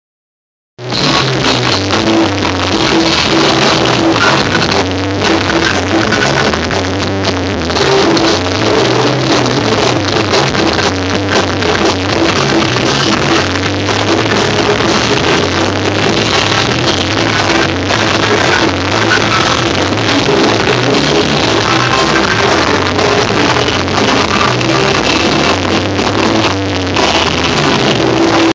Montoursville Little Rock Reunion at The Mill video 3
BAD AUDIO SORRY! I was right in front of the speakers taking video with my phone... This is from the Reunion on September 14th with my dad Ron Lee ROCKING OUT on lead guitar!